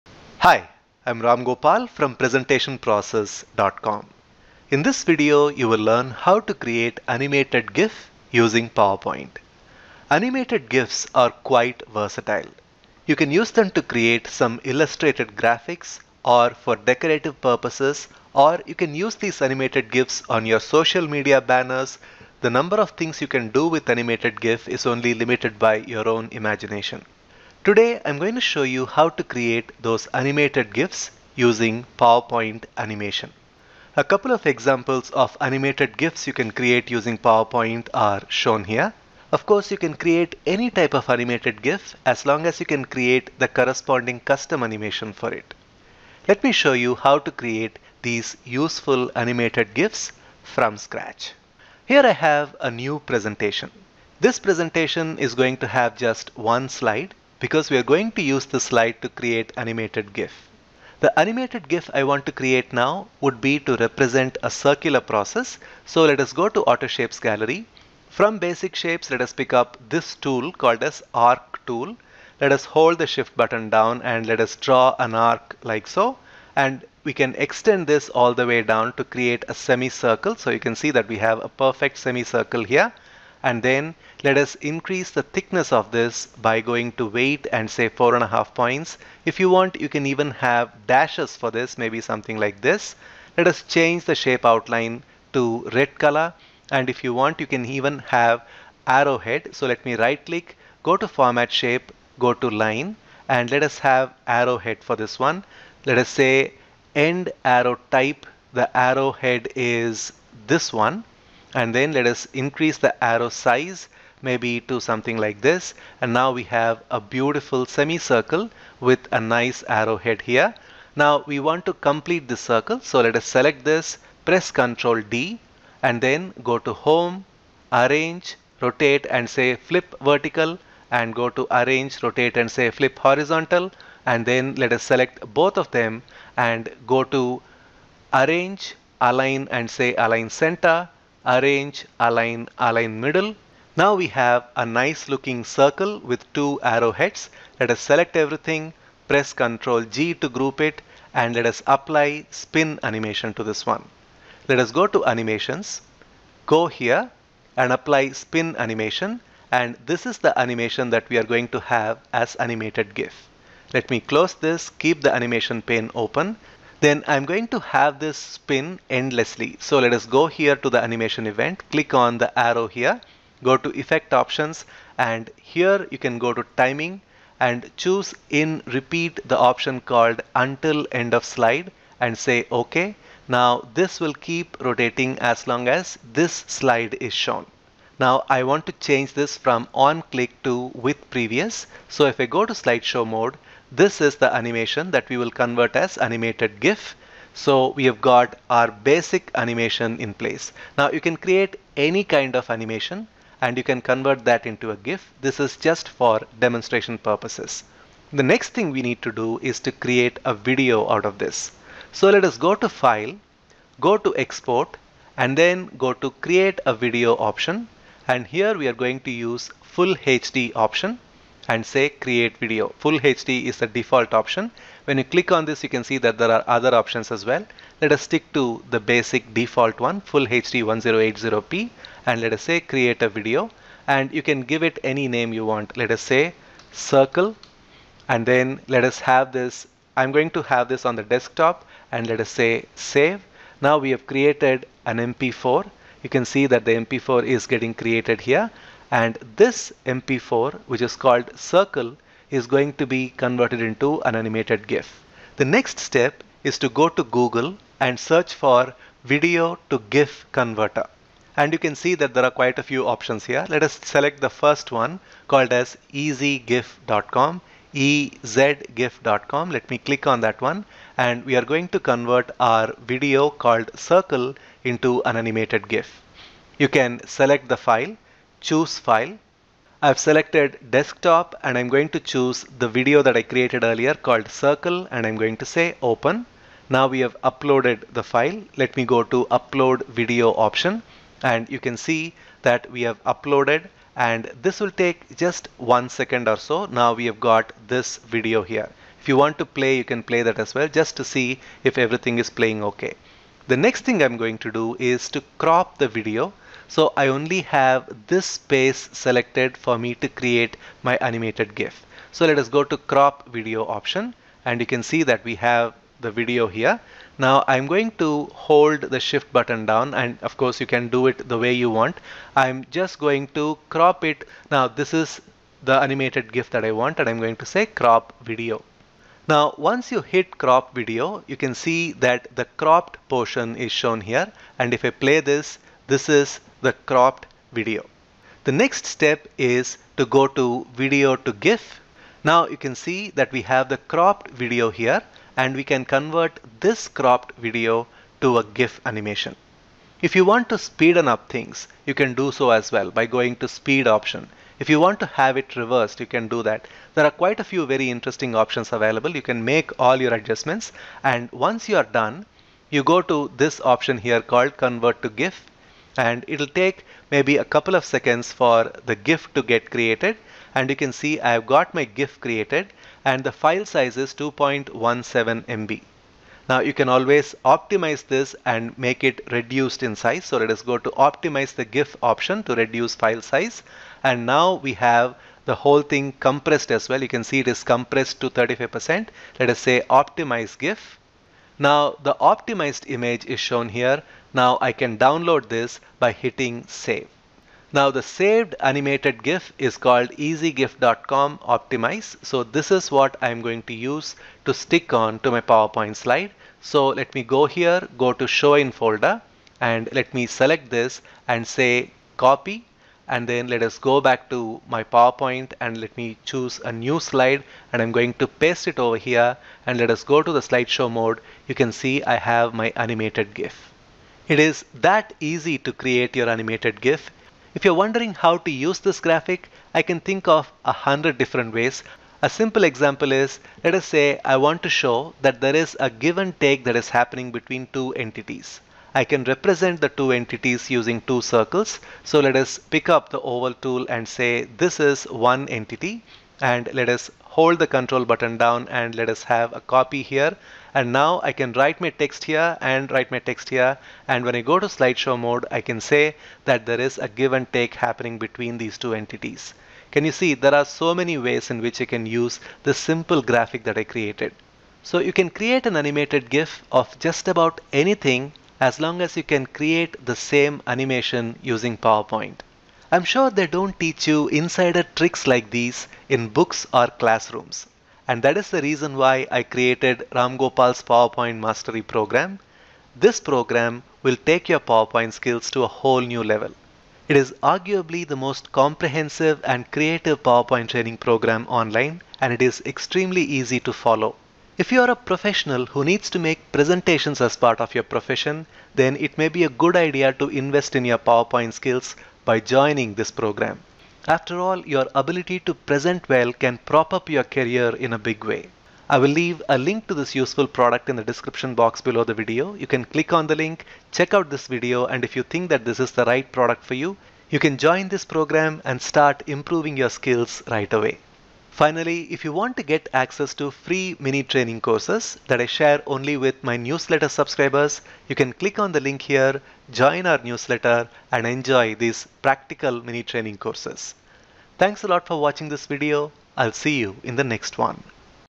How to create Animated GIF using PPT
Learn how to create Animated GIF using PPT. We will use custom animation feature in PowerPoint to create this effect.
Animated GIFs are very versatile and useful image graphics. You can use them in your presentations, in social medial in video creation and more.
We create the basic graphic and animation in PowerPoint and save it the animation as a video. We use an online video to gif converter ( ezgif in this video) to complete the conversion.

Links referred to in this video:
Ramgopal's PowerPoint Mastery Training Program:

Sign Up For Free Mini Training Courses